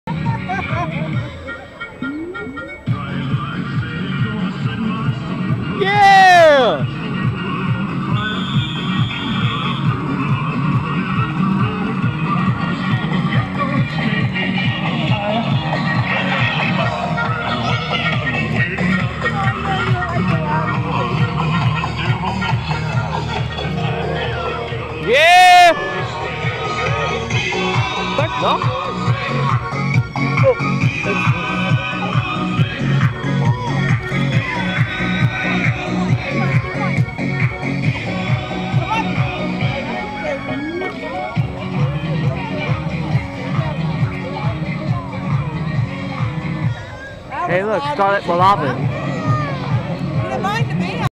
East Manchester Township Homecoming Parade 2014 #6 - 10/2/14
Here is a video of the East Manchester Township homecoming parade of 2014. Part 6 of 11. Enjoy!